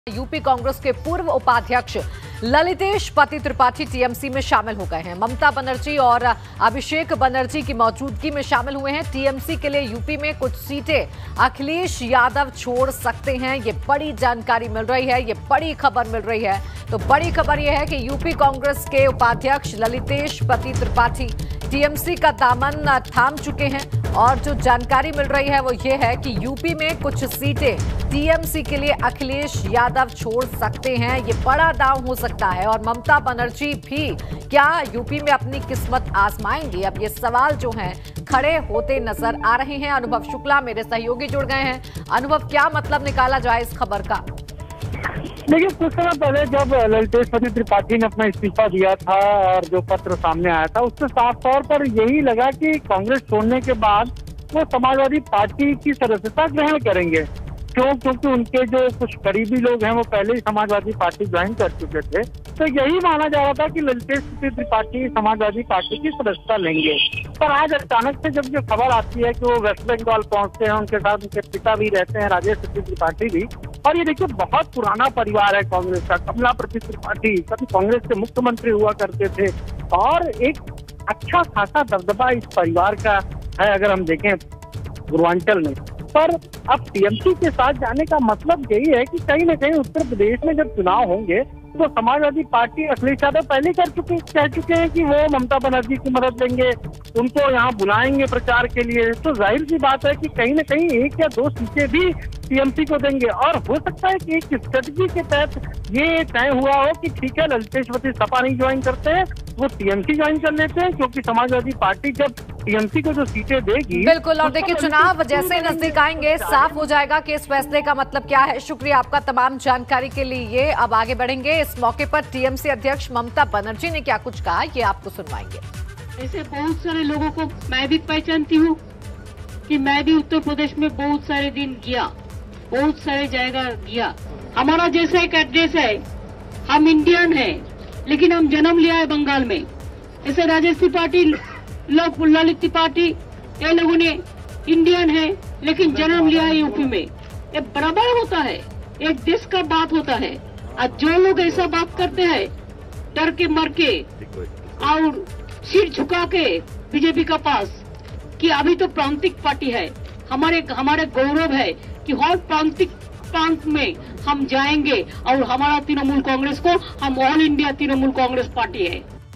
BREAKING NEWS: UP कांग्रेस के पूर्व उपाध्यक्ष ललितेश पति त्रिपाठी TMC में शामिल | UP Politics
यूपी कांग्रेस के पूर्व उपाध्यक्ष ललितेश पति त्रिपाठी TMC में शामिल, ममता बनर्जी और अभिषेक बनर्जी के मौजूदगी में हुए शामिल..